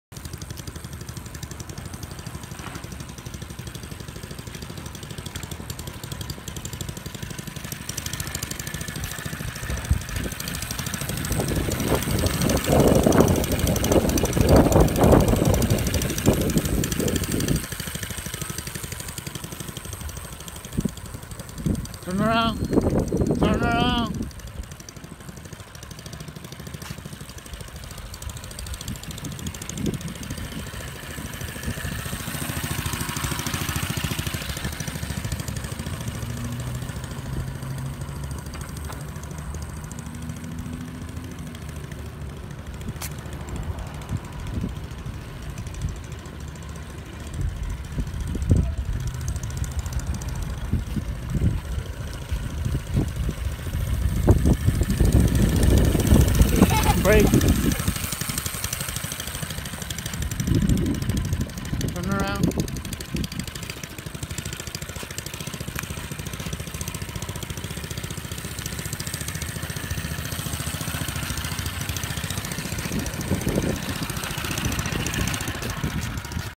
First test razor Mini chopper
My son  project done  doodlebug  minibike motor